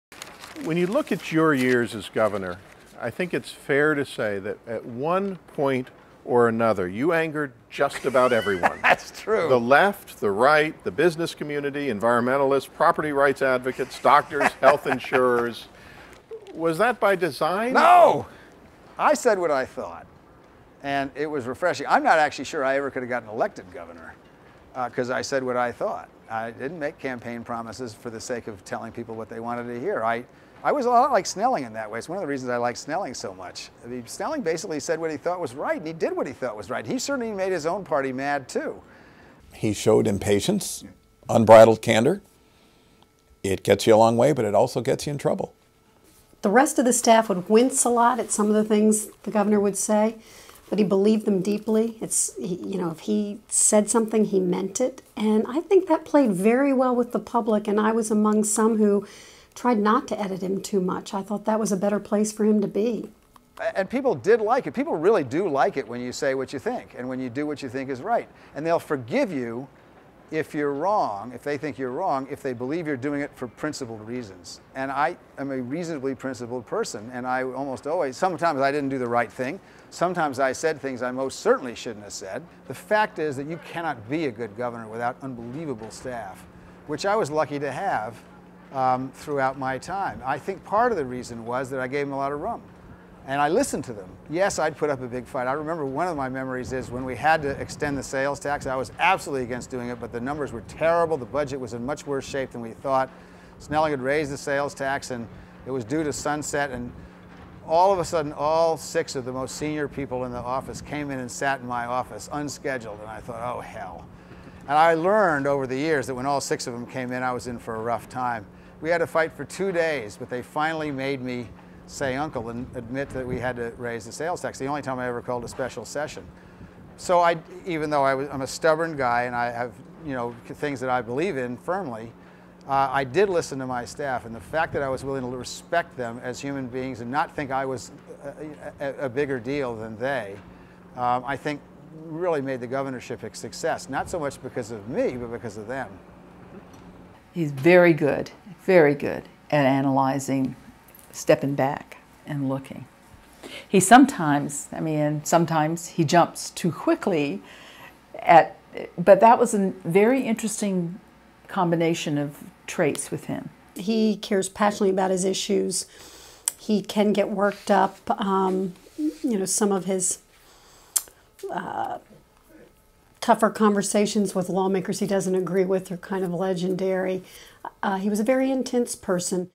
Vermont Governor Howard Dean on Speaking His Mind
Governor Dean always said what he thought, and it sometimes got him in trouble.